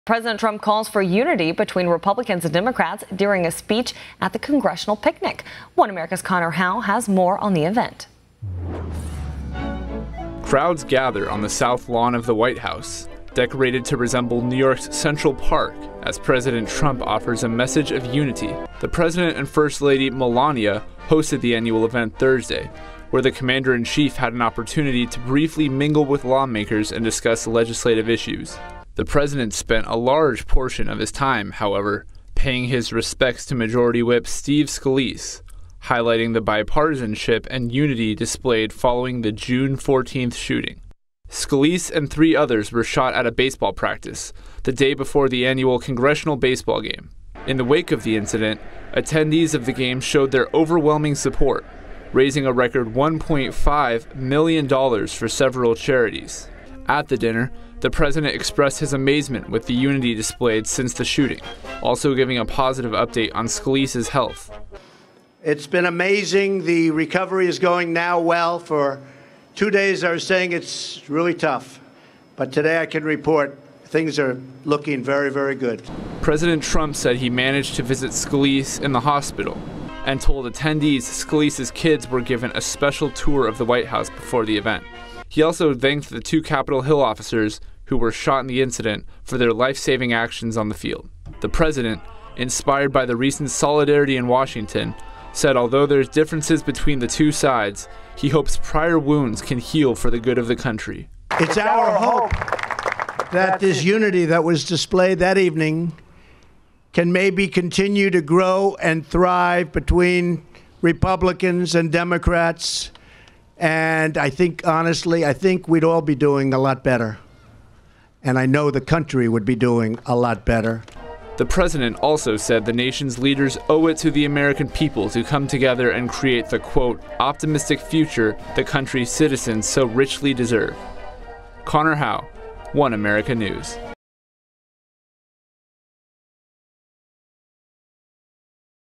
President Trump Calls for Unity at Congressional Picnic
President Trump calls for unity between republicans and democrats during a speech at the congressional picnic.